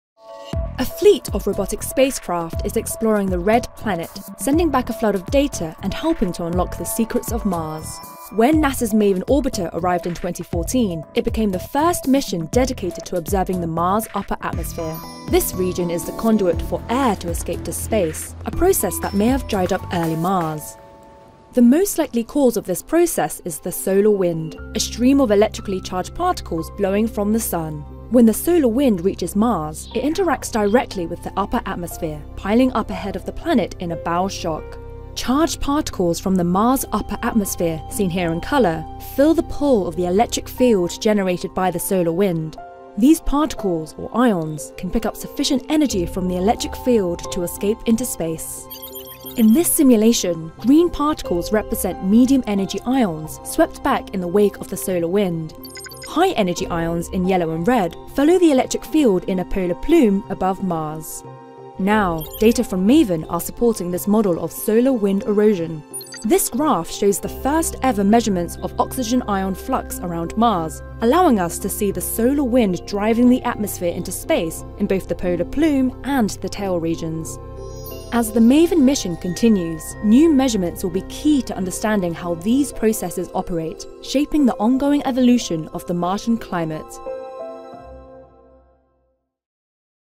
MAVEN | Martian Atmosphere Loss Explained
Scientists have long suspected the solar wind of stripping the Martian atmosphere into space, a process that may have turned Mars from a blue world early in its history into the red planet that we see today. In 2014, NASA's MAVEN orbiter arrived at Mars and began studying its upper atmosphere.

Now, MAVEN has returned the first-ever measurements of solar wind erosion at Mars, observing ions in the upper atmosphere as they pick up energy from the electric field of the solar wind and escape to space.